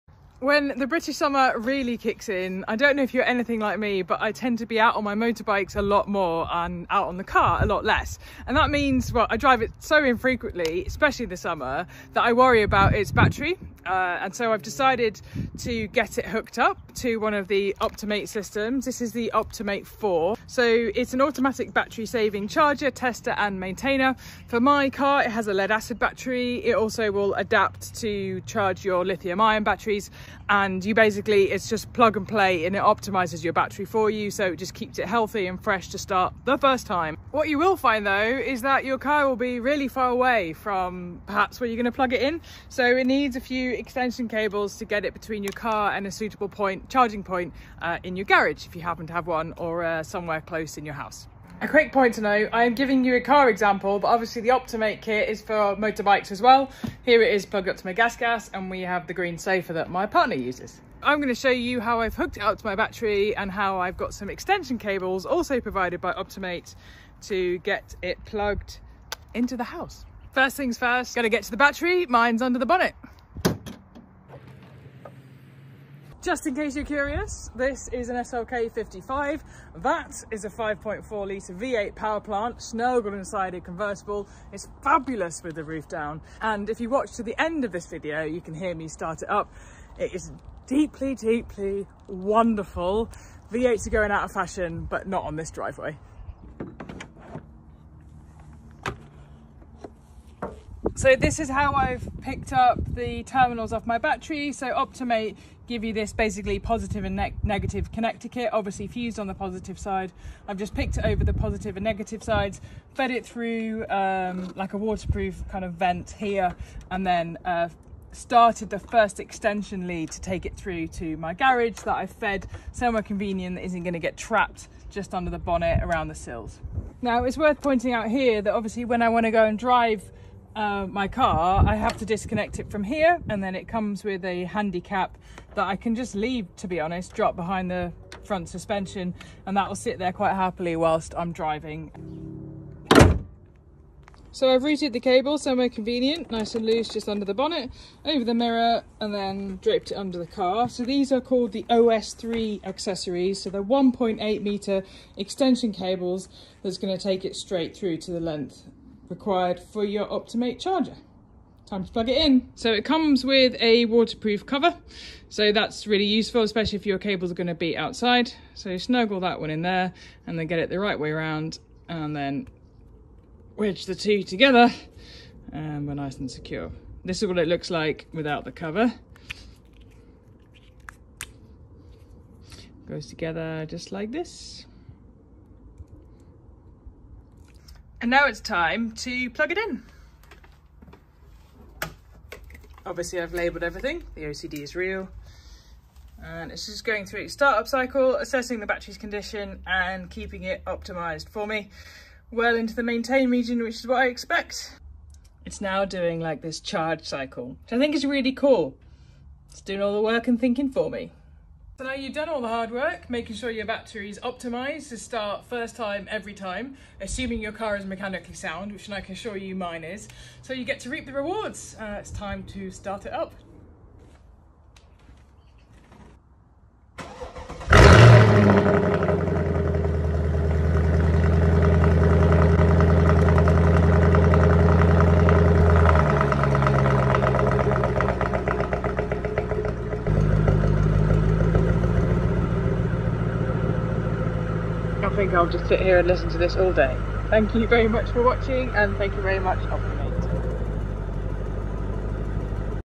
Shows How The OptiMate 4 Can Charge And Maintain Your Car
@Laramoto (racer, engineer and presenter) shows how the OptiMate 4 can charge and maintain your car.